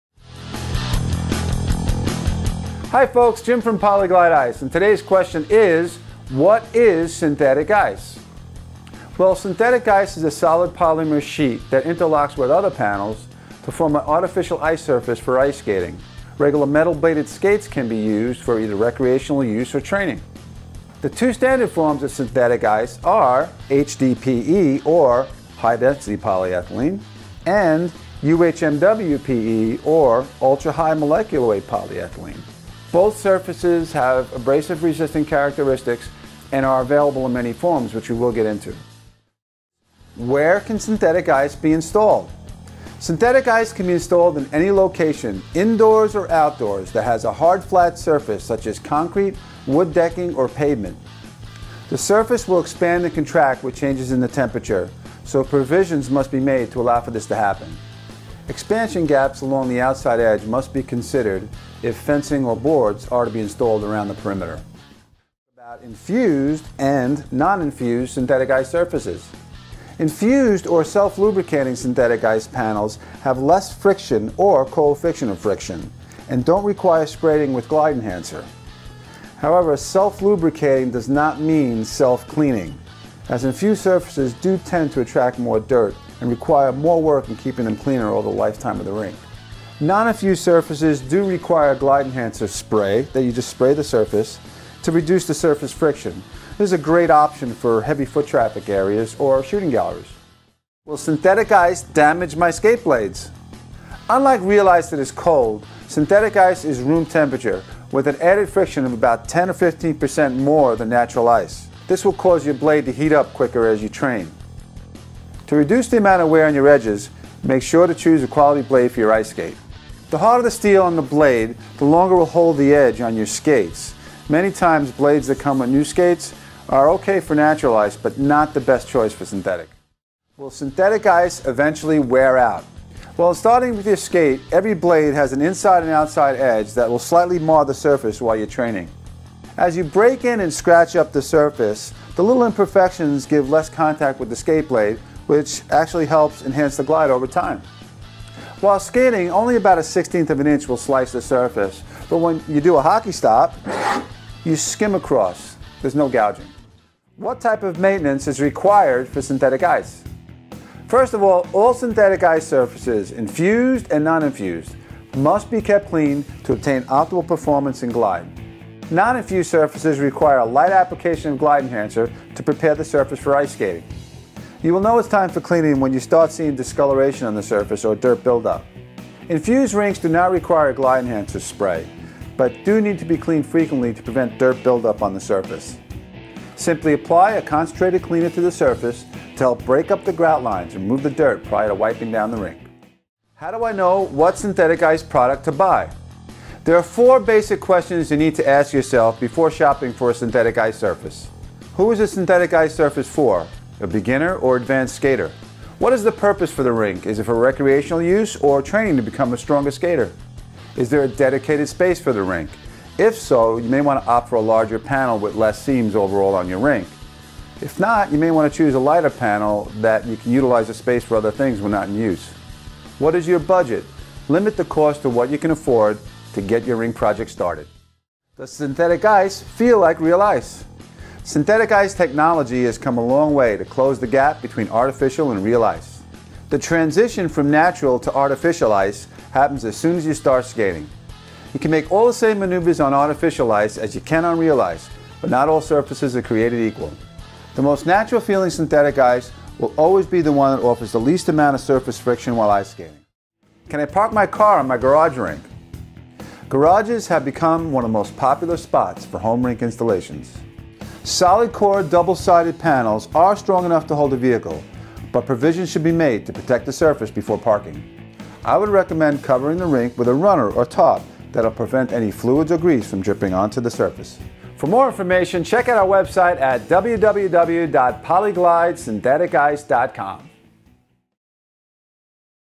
Synthetic Ice - What is Synthetic Ice?- PolyGlide Ice
What is Synthteic Ice? Here's a brief description by Jim Loughran of PolyGlide Ice.